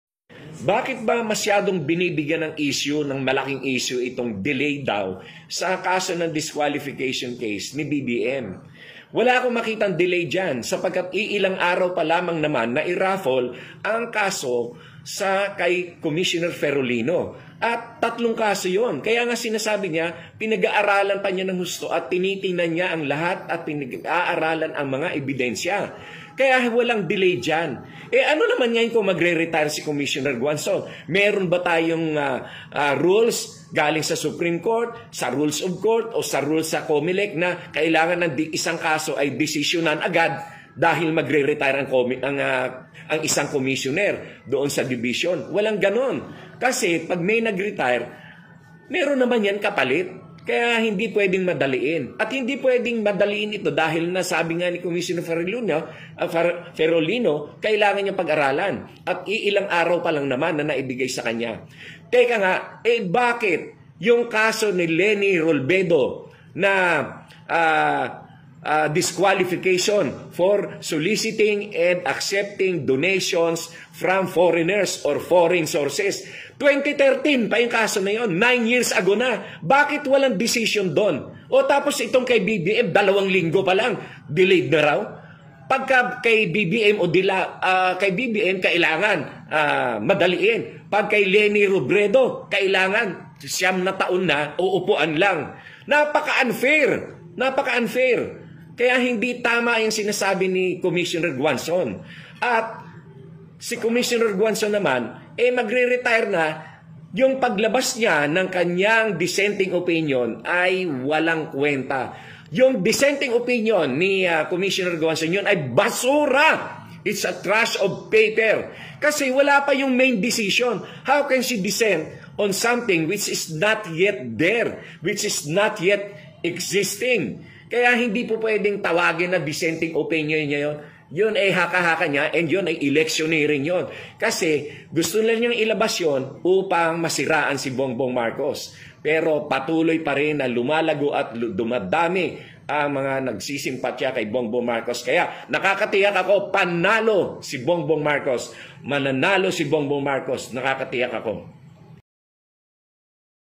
ATTY. LARRY GADON IGINIIT NA WALANG DELAY SA DQ NI BBM | SNO Update
Muling iginiit ni Atty. Larry Gadon na walang delay sa DISQUALIFICATION Case ni BBM tulad ng ipinagdidiinan ng Outgoing Commissioner na si Rowena Guanzon.